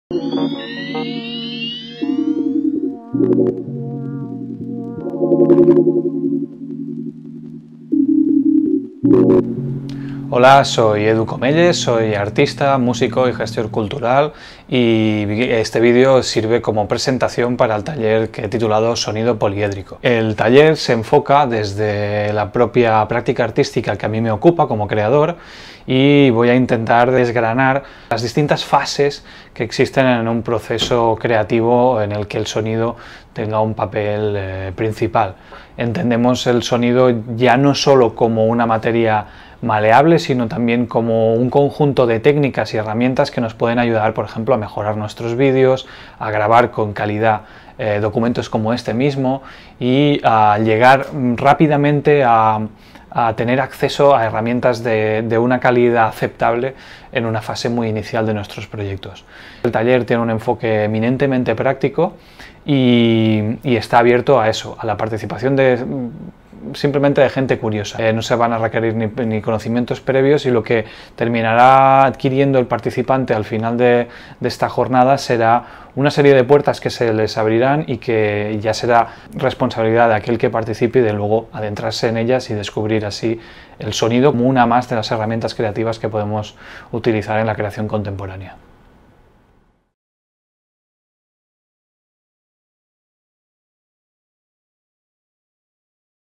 Tráiler taller "SONIDO POLIÉDRICO" con Edu Comelles
Este taller se plantea como un punto de partida para creadores y público generalista interesados en descubrir el sonido como una herramienta y un medio para la creación. El taller se enfocará a través del análisis de distintos ejemplos formales a partir de la obra del autor de la propuesta y desgranará a lo largo de 4 horas distintos contextos, procesos creativos y formalizaciones posibles para adentrarse en una práctica artística centrada en el sonido como materia prima.